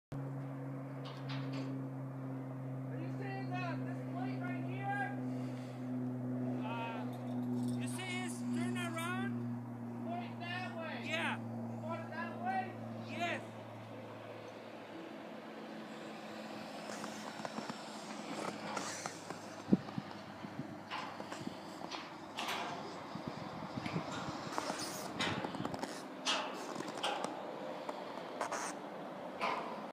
Alberta Transmission Towers near Gibbon
Working on transmission towers in Alberta ,Canada near Edmonton.